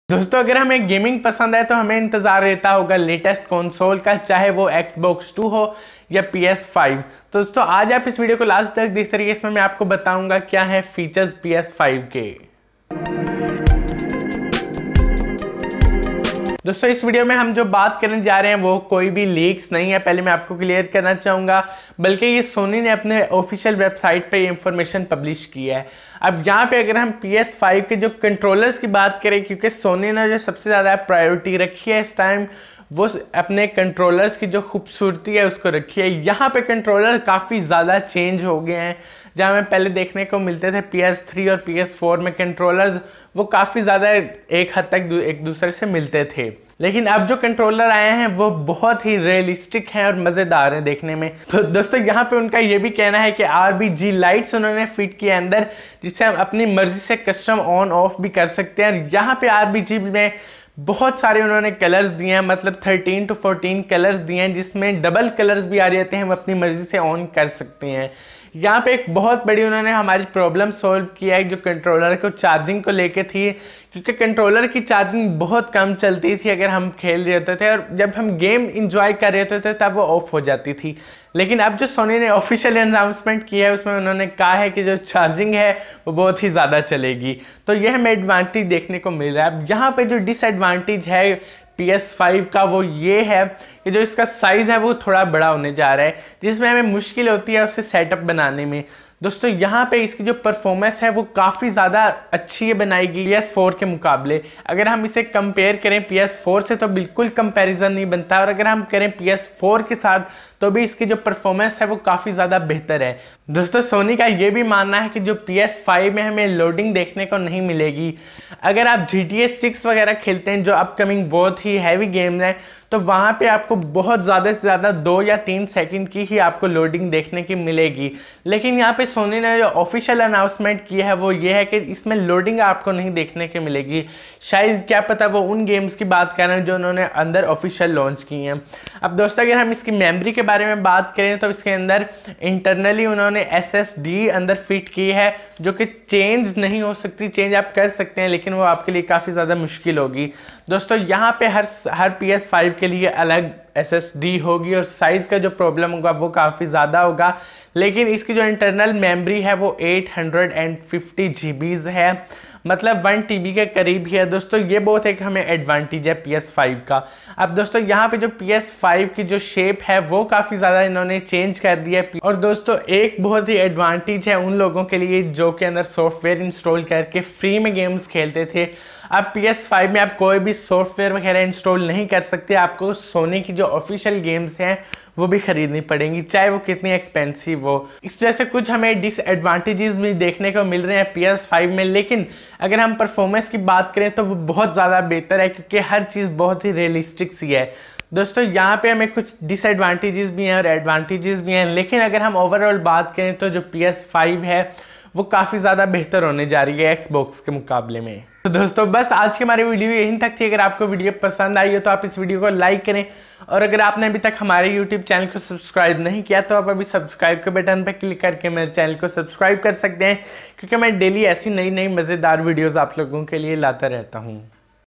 Playstation 5 Specs And Design | Coming soon
This video is about Playstation 5 specs and design and when this Consol is coming in the market. The design of Playstation 5 is amazing as compared to Playstation 4 and Playstation4pro. And if we talk about its specs. The specs are outstanding Sony says that we will not see loading in playstation 5.so thats the good thing.